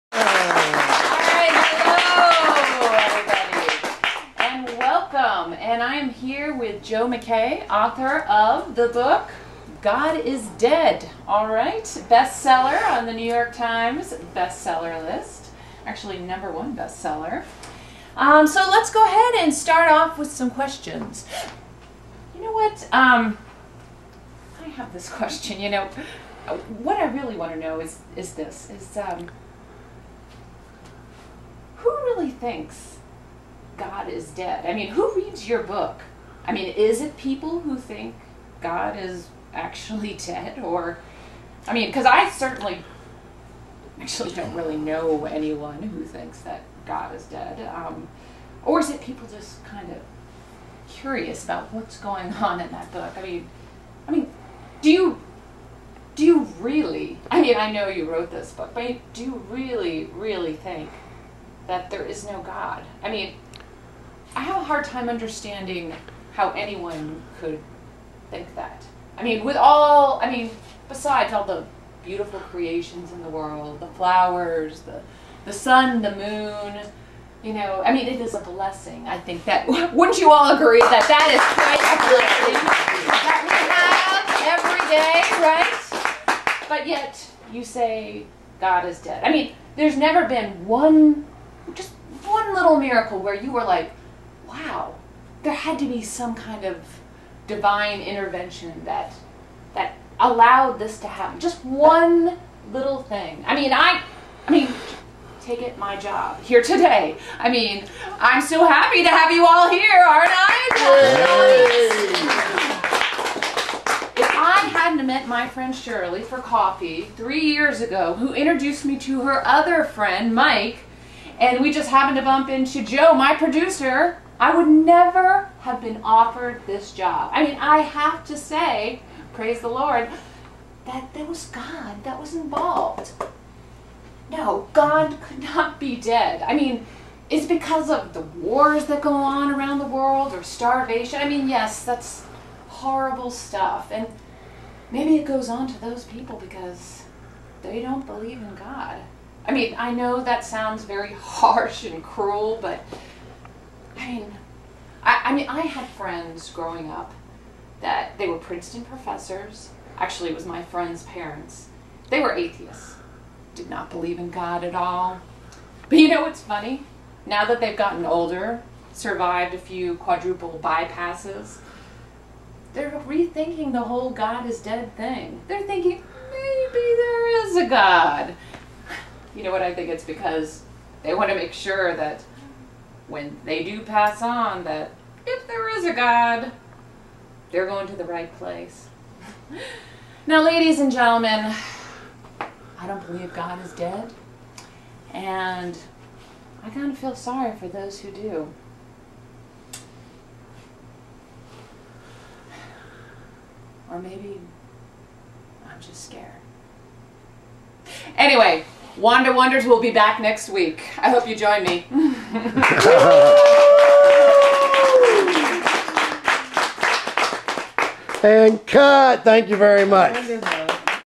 SANDI S. in "God Is Dead?"
A Film Monologue written by Ben Taylor with improv by Sandi

THE BEN TAYLOR WORKSHOP, OCTOBER/NOVEMBER 2014 - THE STUDIO KITCHEN SESSIONS, ATLANTA GA